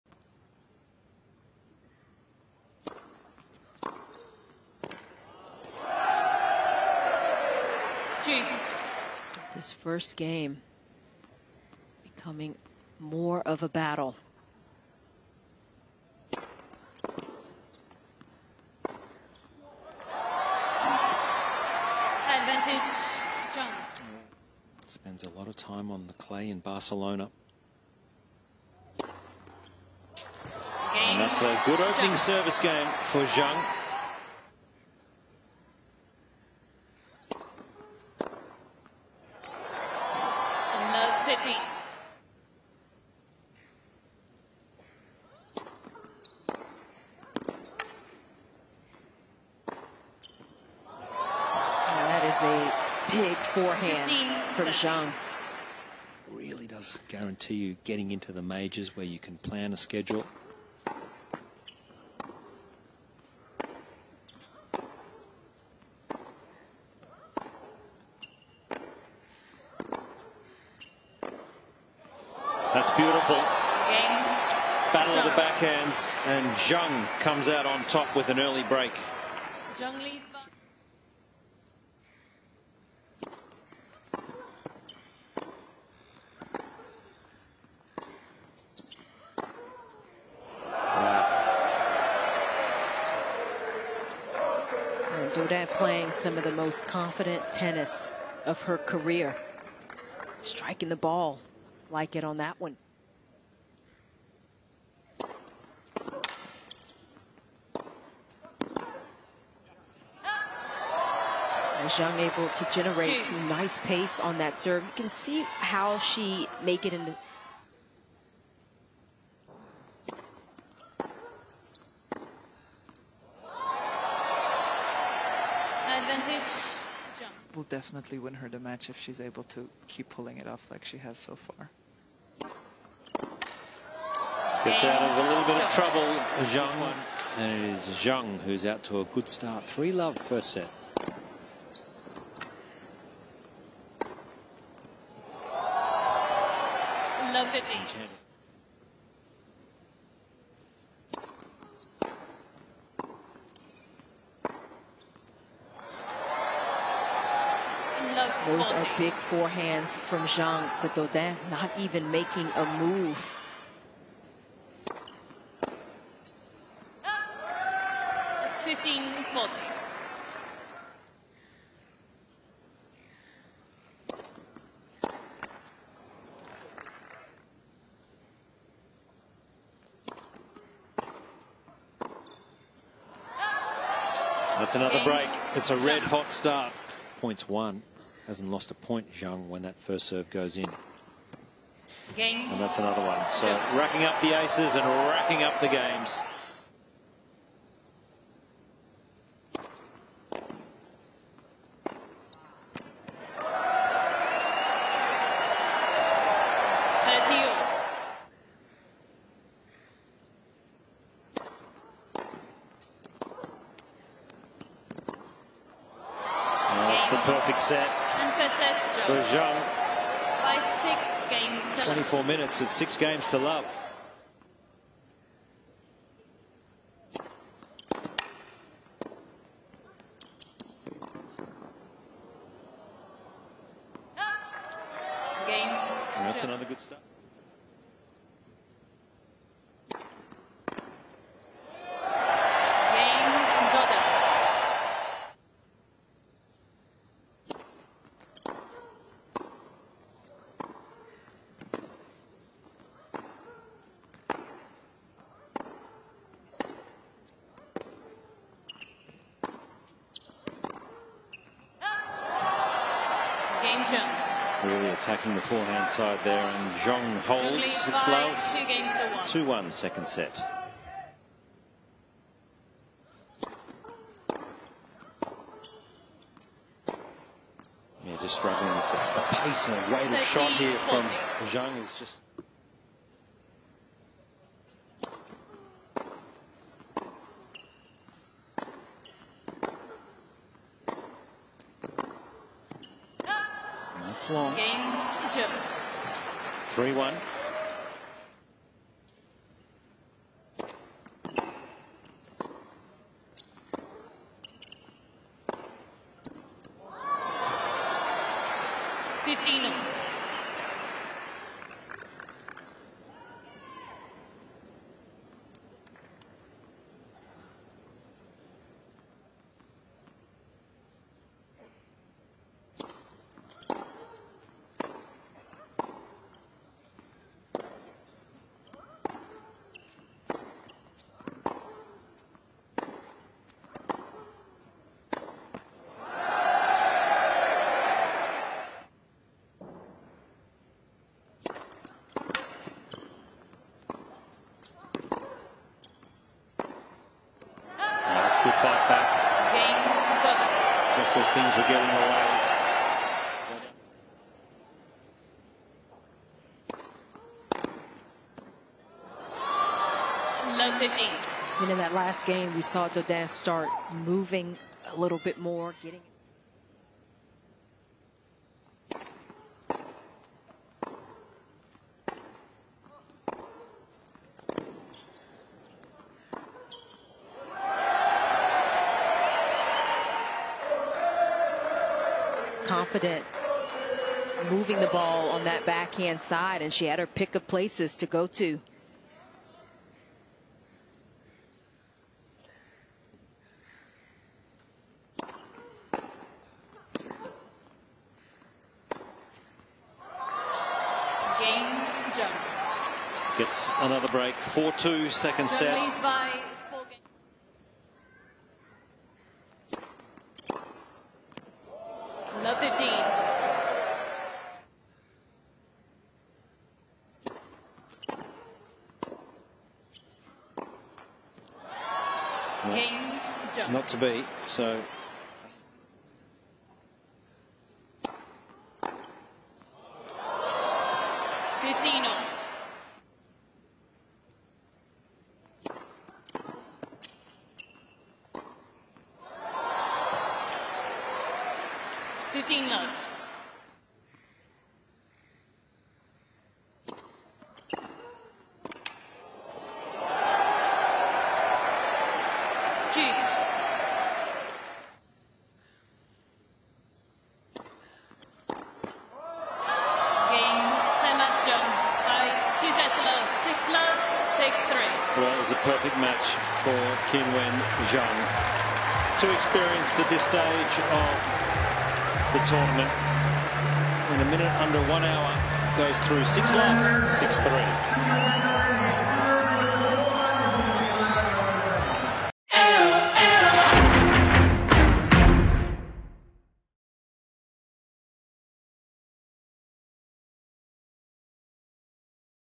Qinwen Zheng v Oceane Dodin Extended Highlights | Australian Open 2024 Fourth Round
Watch the extended highlights of Qinwen Zheng v Oceane Dodin in the Fourth Round of the Australian Open 2024.